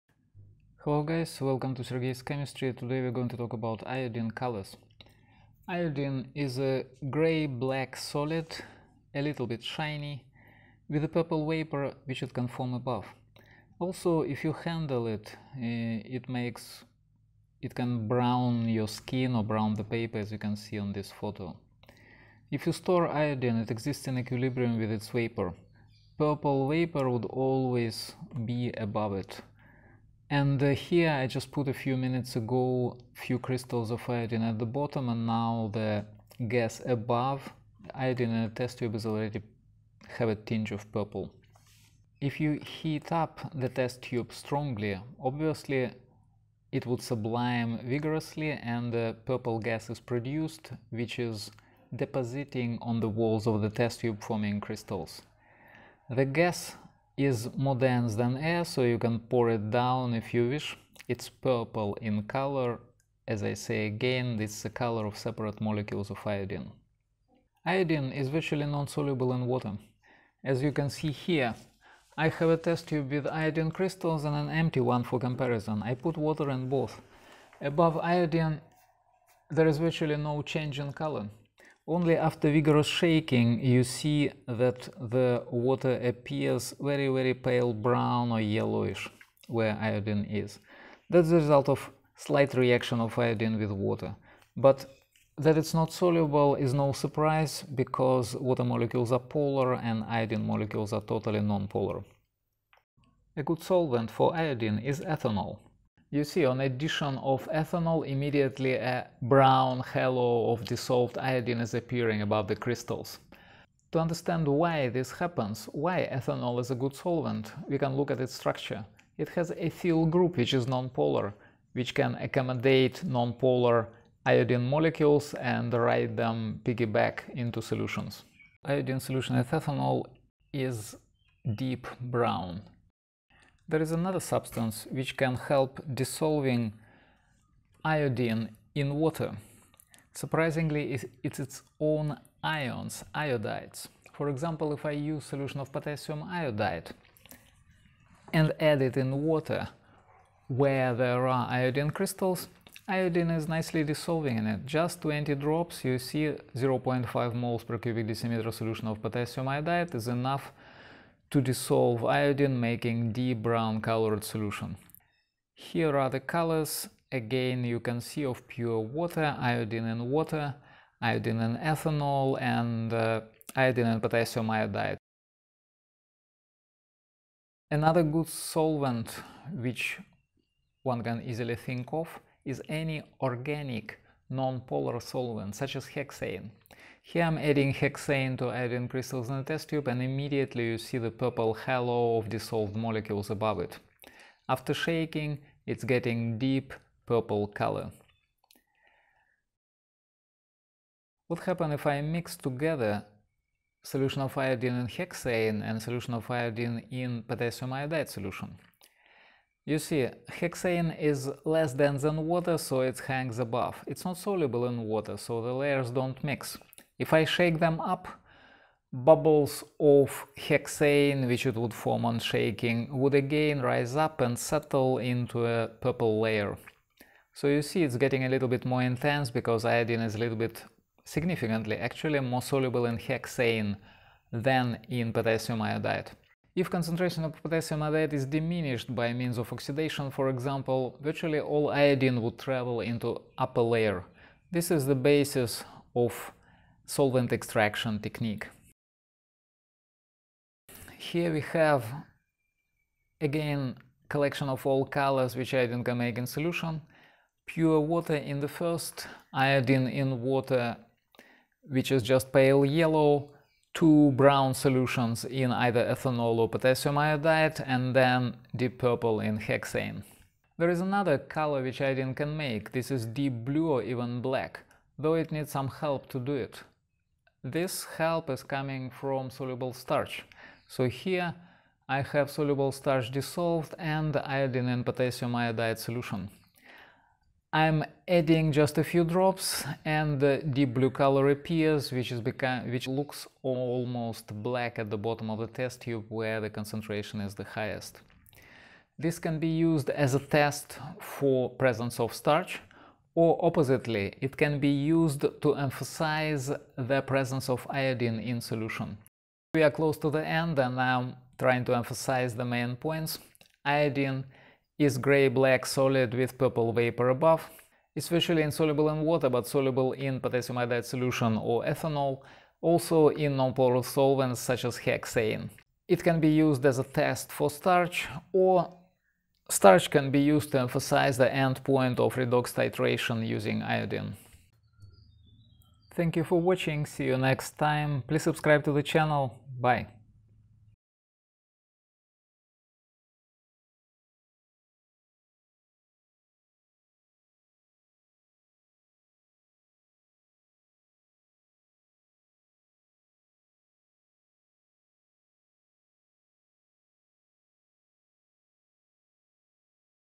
Colors of iodine solutions in water, ethanol, KI (aq) and hexane. Sublimation and starch reaction.
How solid iodine looks and how it sublimes, solutions of iodine in water, ethanol, potassium iodide and hexane.
Explanations are accompanied by high-quality photos.

TUTORING Introductory and Advanced high school Chemistry CBSE, Edexcel,  etc

The colors of iodine:
solid - shiny grey-black
vapor - purple
solution in water - virtually not soluble, yellow or pale brown
in ethanol - brown
in KI(aq) - brown
in hexane - purple
with soluble starch - blue-black

Iodine is not soluble in water as its molecules are nonpolar. It is soluble in ethanol (due to interaction with its nonpolar ethyl group). It is soluble in hexane (which is nonpolar).
It is soluble in aqueous KI due to formation of complexes between iodine molecules and iodide ions.

Iodine reversibly reacts with soluble starch forming deep blue color. In iodine redox titrations soluble starch is added when iodine solution pales to straw color to emphasize the end-point, at which the solution titrated changes from blue to colorless.

Chapters:
00:00 Solid iodine appearance
00:22 Gaseous iodine in equilibrium with solid
00:42 Sublimation of iodine
01:07 Solubility of iodine in water
01:43 Solubility of iodine in ethanol
02:13 Solubility of iodine in iodide solutions (KI (aq))
02:57 Solution of iodine in hexane (in alkanes)
03:21 Distribution of iodine in layers of hexane and KI (aq); solvent extraction
04:10 Overview of solutions of iodine in different solvents
04:30 Iodine and starch reaction
05:09 Main points of the video revisited